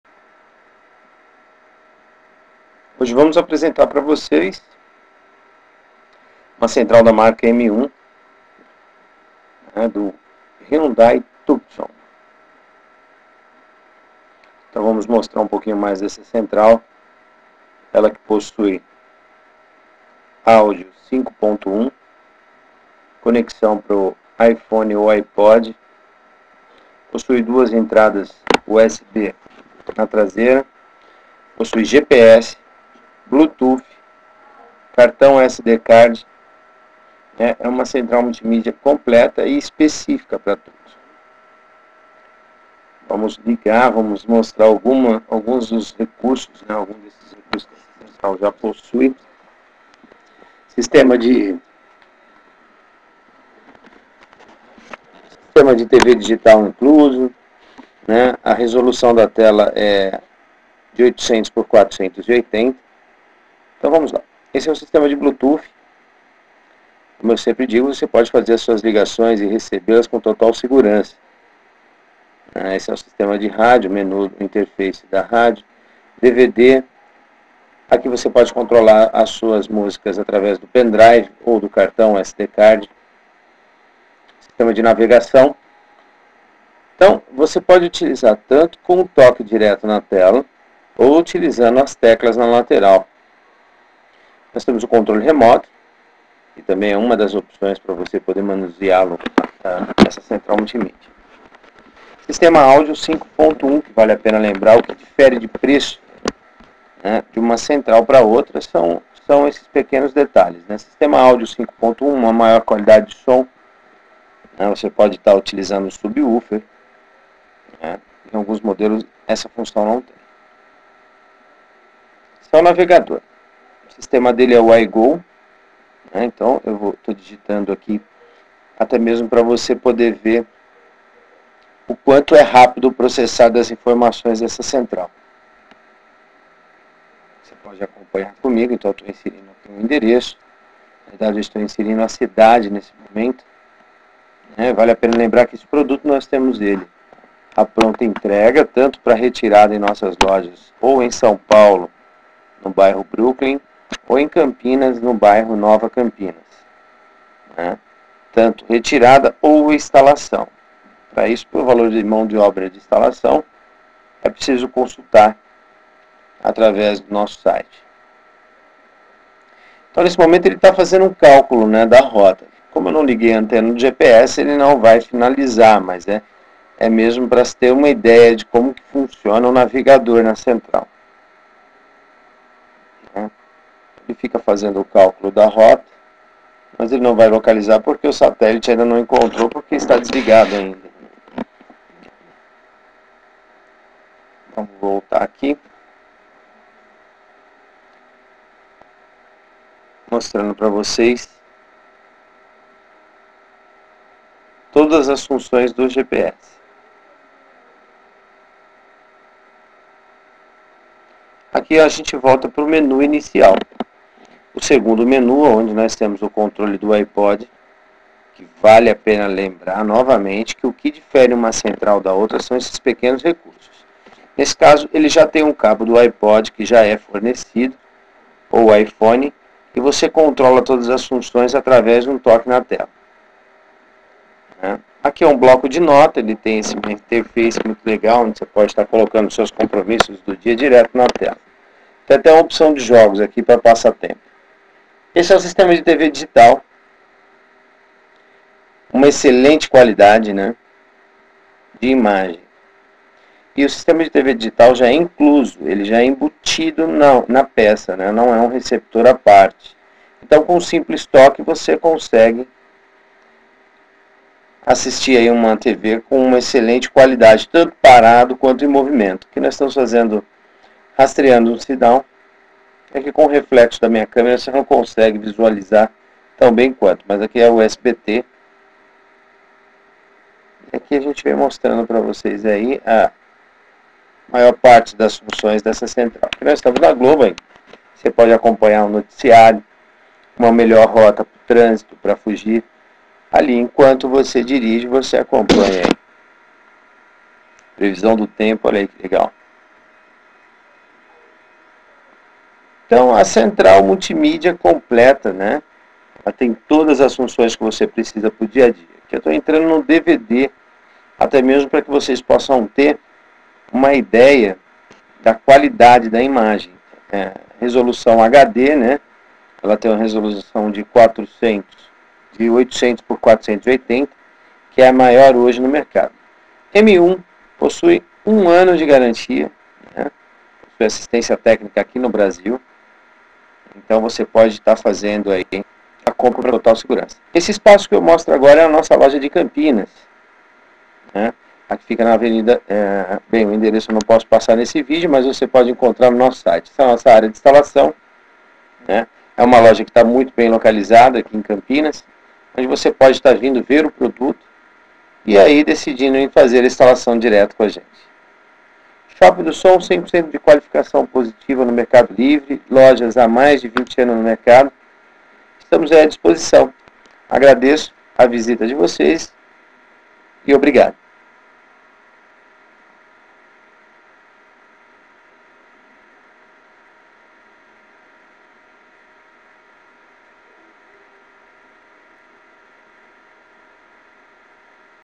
CENTRAL MULTIMIDIA TUCSON TV DIGITAL TELA HD
DVD PlayerSintonizador de TV Digital integrado, com comando pela tela
Navegador GPS IGO8 com 1369 cidades mapeadas, 311 delas totalmente auditadas
Menus e orientação por voz em português
Reproduz DVD/DVD-R/DVD-RW/VCD/CD/CD-R/MP3/MP4/Divx/Jpeg/Gigamp3 (DVD-MP3)
Função Dual zone - Você pode ouvir música, ou assistir DVD enquanto navega no GPS ou vice-versa
Tela touch screen 7 polegadas widescreen TFT 262.144 cores (alta definição de imagem)
Entrada USB (pendrive, MP3)
100% compatível com os controles do volante
Controle de Ipod diretamente na tela - cabo incluso
Bluetooth viva-voz - Receba e efetue ligações de seu celular direto pela tela do aparelho ) - Identificador de chamadas na tela
Áudio streaming (A2DP) -- reproduz arquivos de áudio em estéreo de seu celular ou Mp3 player
200 watts de potência
Saída de áudio 5.1 com canal central e subwoofer
Função equalizador gráfico com 13 bandas
Controle remoto multifunção
Entrada áudio/vídeo auxiliar
Sensor de marcha ré para ativação automática de câmera traseira
Conexão com sensor de estacionamento (opcional) com funcionamento juntamente com a câmera de ré
Sensor de freio de mão para reprodução de vídeo (para atender as exigências do Contran)
Instalação super simples. Utiliza os mesmos encaixes e chicotes do som original de seu carro, sem nenhum corte ou adaptação
Função 3zone - Você pode utilizar uma função na tela principal e outra diferente nas telas traseiras]
Tela digital de alta resolução (HD - High Definition) com 800x480 pixels, assegurando maior riqueza dos detalhes, brilho, nitidez e clareza com qualquer iluminação.
DVD com função touch screen, escolha a música tocando diretamente no menu que aparece na tela.
Leitura rápido do disco, em 5-6 segundos
Suporta DVD/VCD/CD/MP3/MPEG4/CD-R/WMA/JPEG.
Armazenamento de memória para USB,SD,CD depois de desligar.
Circuito de DVD otimizado, economia no consumo de energia, estabilidade elevada.
Zoom in ou zoom out diretamente pela tela De um a quatro níveis ajustáveis
Disque diretamente da tela, grave os números discados e recebidos, aumente a segurança ao volante.
Bluetooth music player
Compatível com iPod
Opere seu iPod diretamente da tela do aparelho, Toque Mp3/Mp4, veja fotos e vídeos
Dual zone
Algumas funções podem ser usadas simultaneamente
Incluindo opções de visualização, estacionamento, sensor de luz,
GPS, música etc.